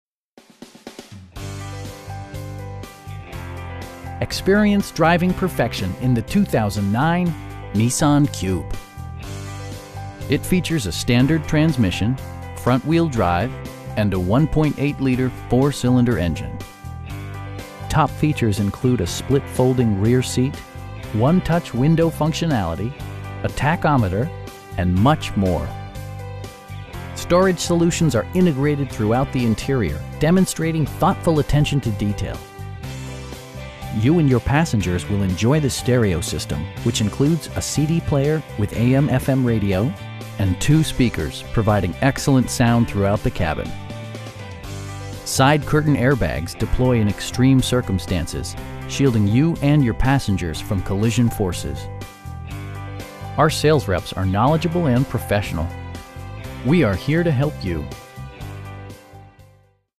2009 Nissan Cube 1.8 in Salt Lake City, UT 84111
Experience driving perfection in the 2009 Nissan Cube. It features a standard transmission, front-wheel drive, and a 1.8 liter 4 cylinder engine. Top features include a split folding rear seat, 1-touch window functionality, a tachometer, air conditioning, tilt steering wheel, power door mirrors, power windows, and much more. Storage solutions are integrated throughout the interior, demonstrating thoughtful attention to detail. You and your passengers will enjoy the stereo system, which includes a CD player with AM/FM radio, and 2 speakers, providing excellent sound throughout the cabin. Side curtain airbags deploy in extreme circumstances, shielding you and your passengers from collision forces. Our sales reps are knowledgeable and professional. We'd be happy to answer any questions that you may have. We are here to help you.